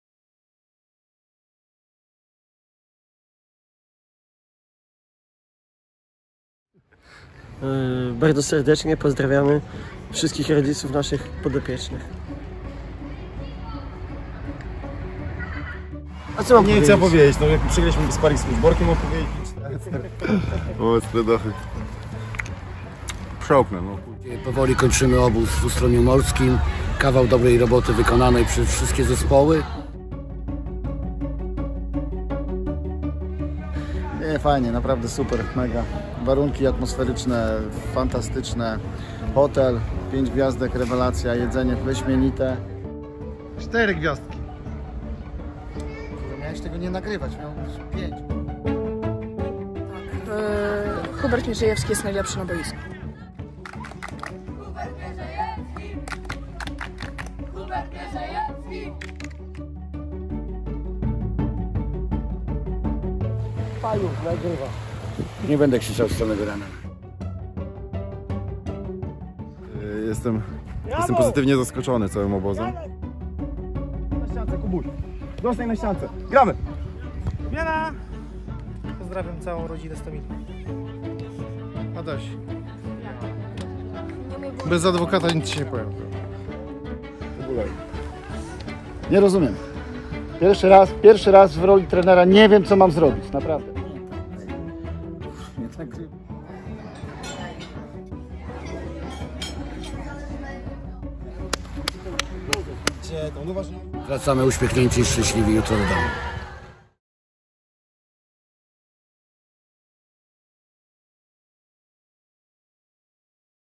Obóz Ustronie Morskie 2022r.
Podsumowanie obozu na wesoło i sportowo :)